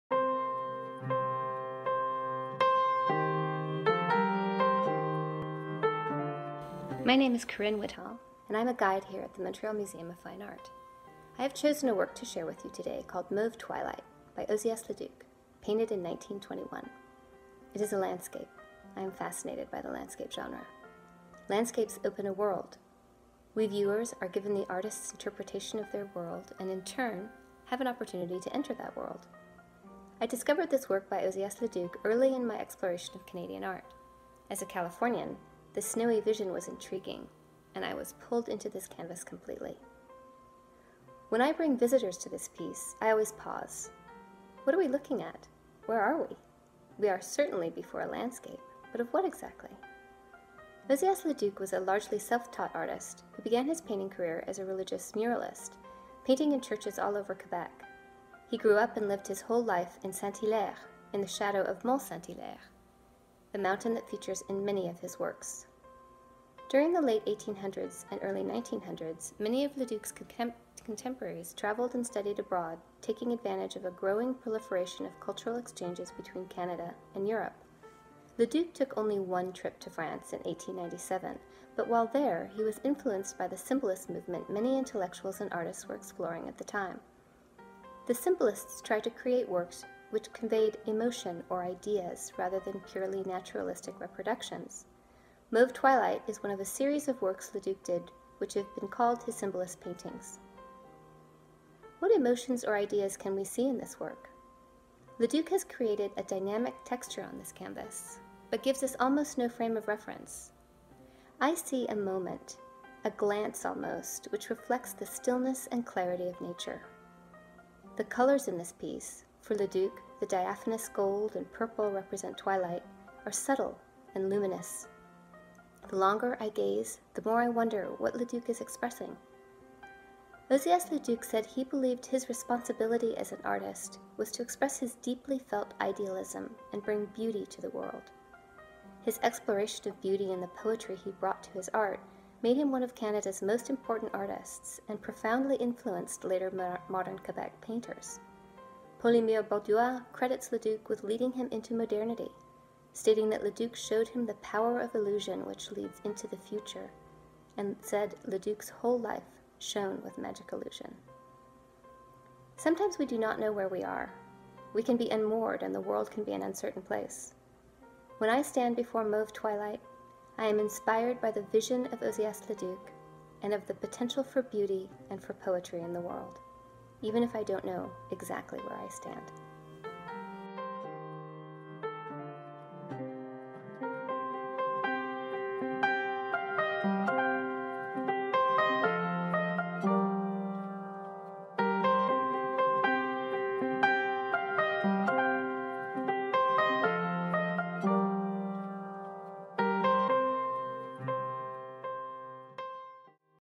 3 minutes avec une œuvre d'art | with an artwork
💬 Au quotidien, les guides bénévoles du MBAM donnent de leur temps afin de transmettre leur passion pour l’art au plus grand nombre.

📽 Lorsqu’il ne leur est pas possible de vous guider dans les salles du Musée, c’est virtuellement qu’elles vous dévoilent les détails et mystères qui se cachent dans les œuvres de nos collections.

Ozias Leduc, «L’heure mauve», 1921.
MBAM, achat, don de Mme Samuel Bronfman à l’occasion du 70e anniversaire de son mari.
© Succession Ozias Leduc / SOCAN (2020)

💬 On a daily basis, The MMFA’s Volunteer Guides give their time to share their passion for art with as many people as possible.

📽 When unable to show you around in person, they take to your screens to shed light on the details and mysteries hidden throughout our collections.

Ozias Leduc, “Mauve Twilight,”1921.
MMFA, purchase, gift of Mrs. Samuel Bronfman in honour of the 70th birthday of her husband.
© Estate of Ozias Leduc / SOCAN (2020)